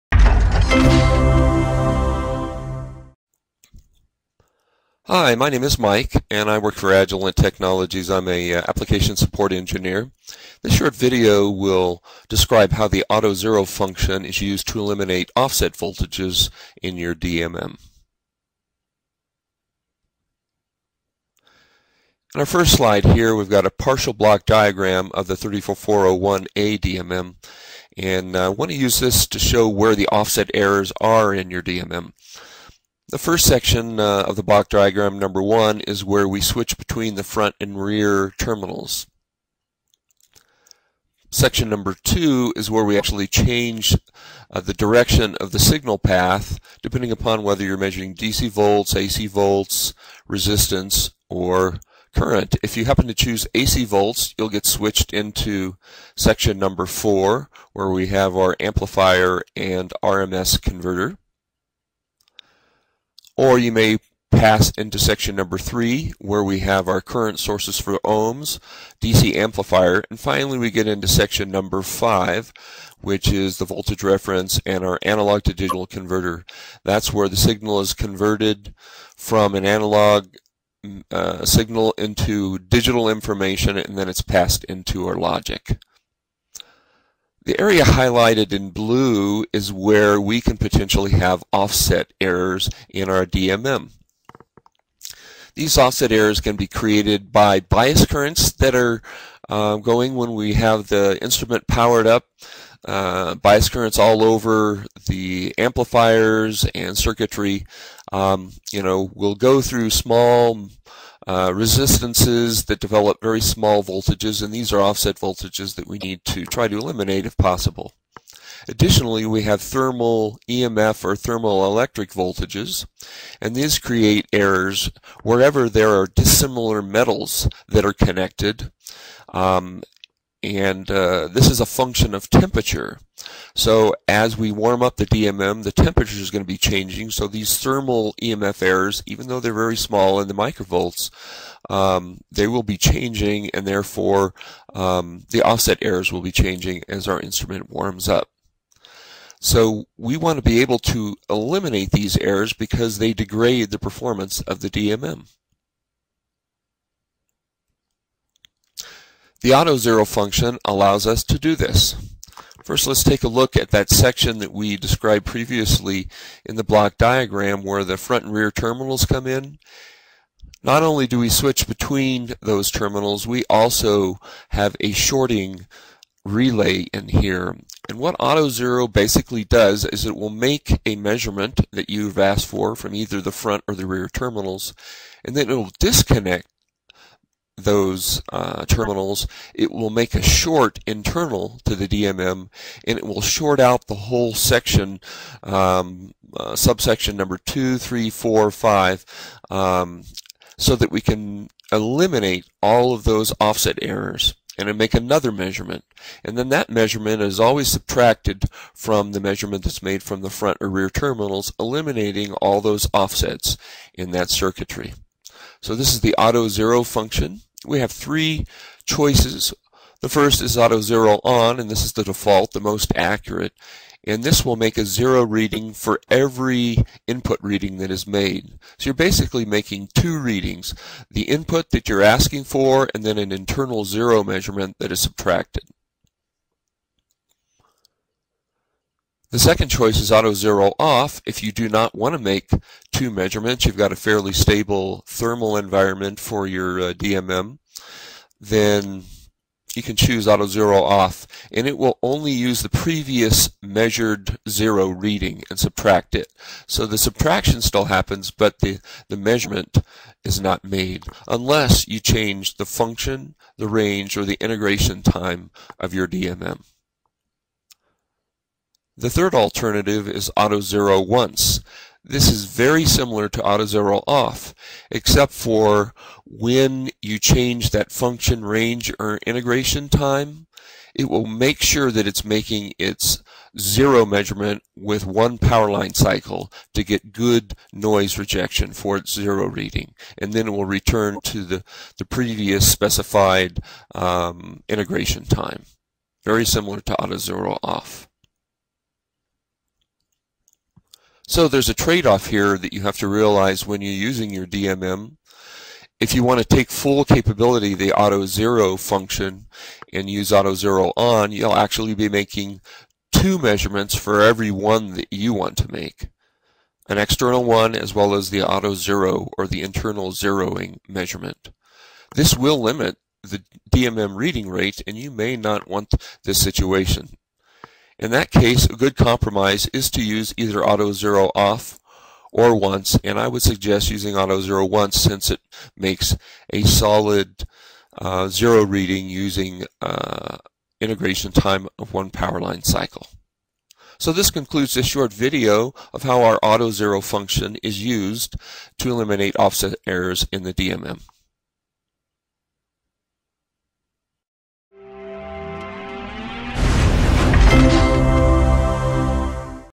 Understanding the AUTOZERO function in Digital Multimeter DMMs
Describes how the AUTOZERO function elimiinates offset voltages in Digital Multimeters DMMs.